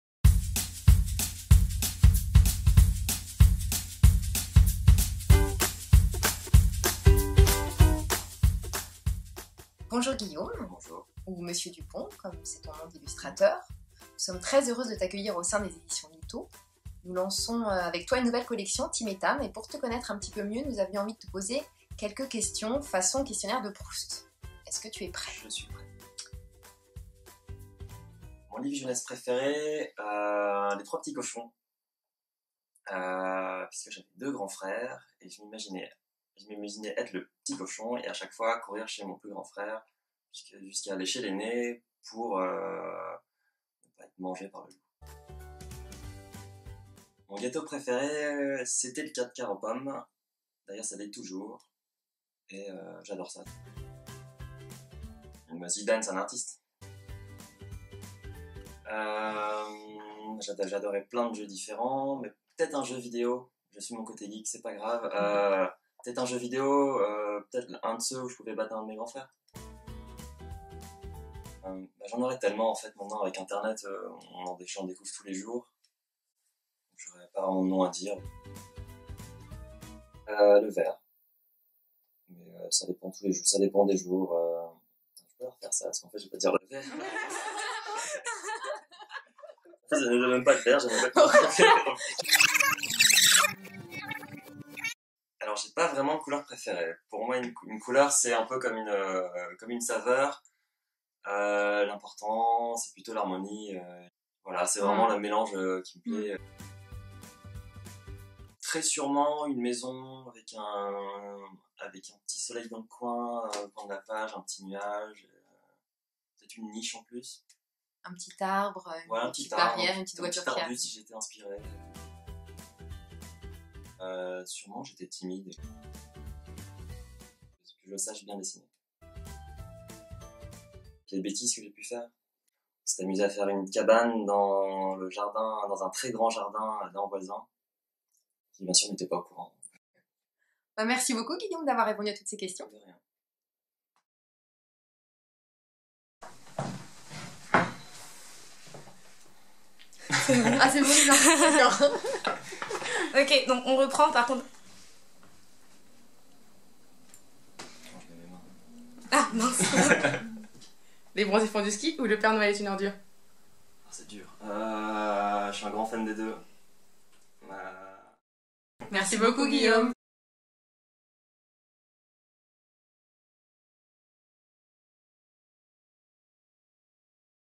Le questionnaire de Proust de MONSIEUR DUPONT
Pour la sortie de la nouvelle collection "TIM et TAM partent à l'aventure !", nous vous proposons de faire la connaissance de l'illustrateur Monsieur Dupont.